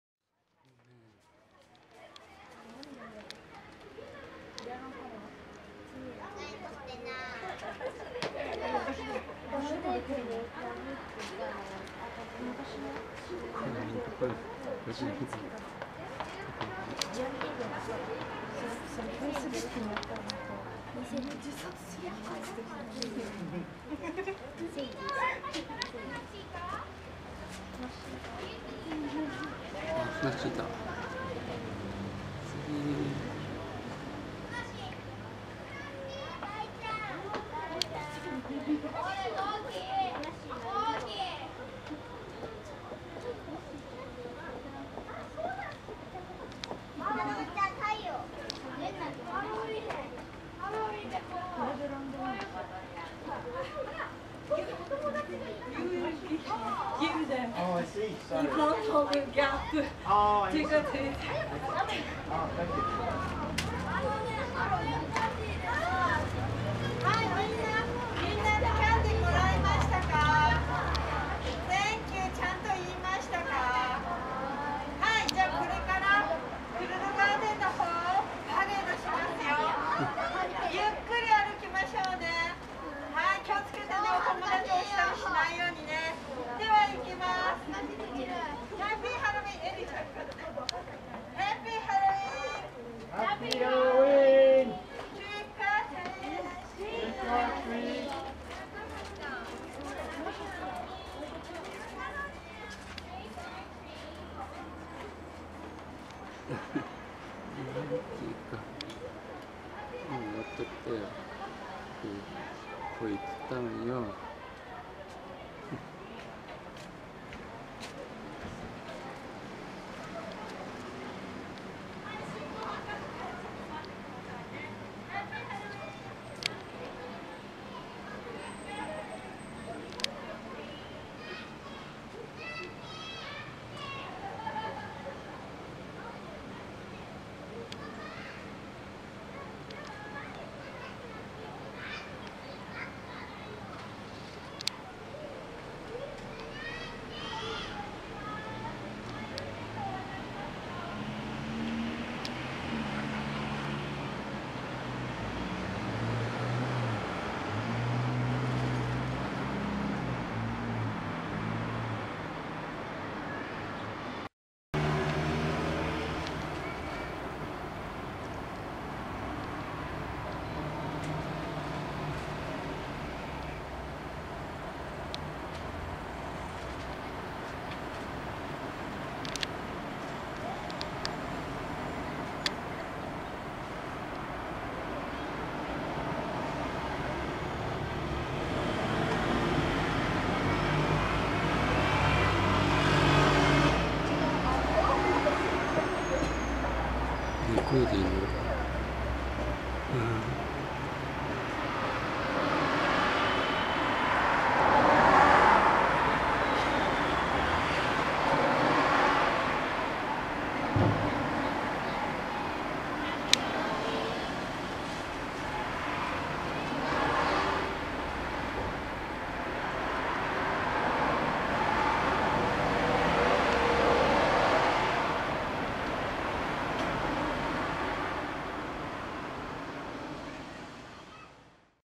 アメリカンランゲージ 2014年 ハロウイン 10月26日
アメリカンランゲージ　2014年のハロウインパーティの様子です。
街の中をみんなでねり歩きます。